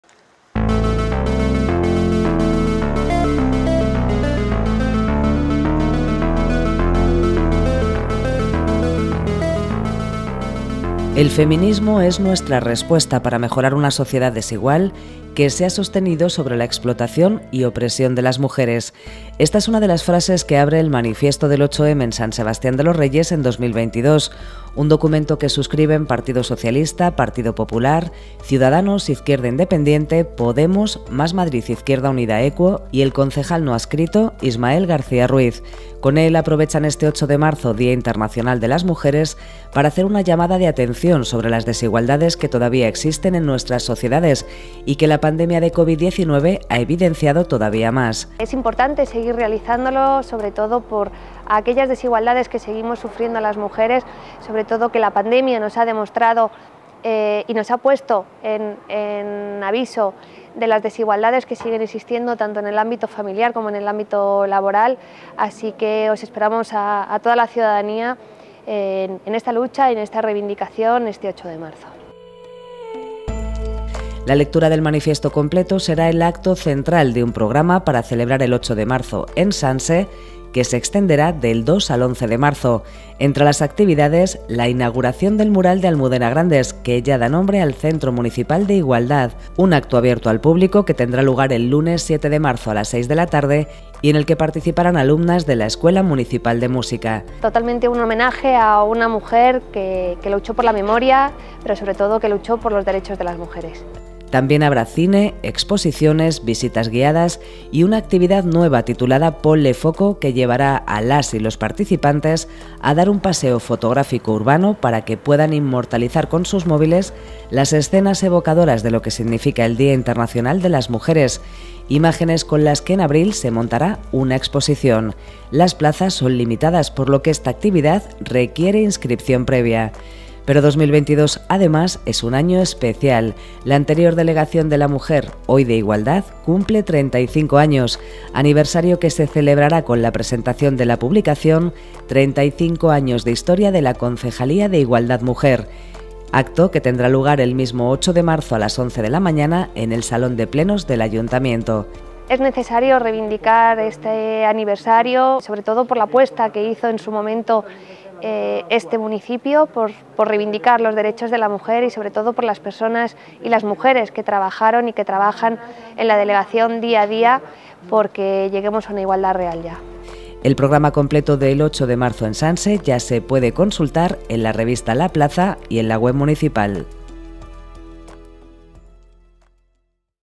Sanse celebra los éxitos y aportaciones del feminismo el próximo 8M, Día Internacional de la Mujer
«El feminismo es nuestra respuesta para mejorar una sociedad desigual que se ha sostenido sobre la explotación y opresión de las mujeres». Esta es una de las frases que abre el Manifiesto del 8M de San Sebastián de los Reyes en 2022, un documento que suscriben PSOE, PP, Ciudadanos, Izquierda Independiente, Podemos, Más Madrid Izquierda Unida Equo y el concejal no adscrito Ismael García Ruiz. Con él, aprovechan este 8 de Marzo, Día Internacional de las Mujeres, para hacer una llamada de atención sobre las desigualdades que todavía existen en nuestra sociedad y que la pandemia de COVID-19 ha evidenciado todavía más.

La lectura del manifiesto completo (martes, 8 de marzo de 2022, Plaza de la Constitución, 12.00 h.), será el acto central de un programa para celebrar el 8 de Marzo en Sanse que se extenderá del 2 al 11 de marzo.

Entre las actividades, la inauguración del mural de Almudena Grandes que ya da nombre al Centro Municipal de Igualdad, acto abierto al público que tendrá lugar el lunes 7 de marzo, a las 18.00 h. Y en el que participarán alumnas de la Escuela Municipal de Música.

También habrá cine, exposiciones, visitas guiadas y una actividad nueva titulada «Ponle foco» que llevará a las y los participantes a un paseo fotográfico urbano para que puedan inmortalizar con sus móviles las escenas evocadoras de lo que significa el Día Internacional de las Mujeres, imágenes con las que en abril se montará una exposición. Las plazas son limitadas, por lo que esta actividad requiere inscripción previa.

Pero 2022, además, es un año especial, la anterior Delegación de la Mujer, hoy de Igualdad, cumple 35 años, aniversario que se celebrará con la presentación de la publicación “35 años de historia de la Concejalía de Igualdad-Mujer”, el mismo 8 de marzo a las 11.00 h., en el Salón de Plenos del Ayuntamiento.

El programa completo del 8M en Sanse se puede consultar en la revista La Plaza y en la web municipal.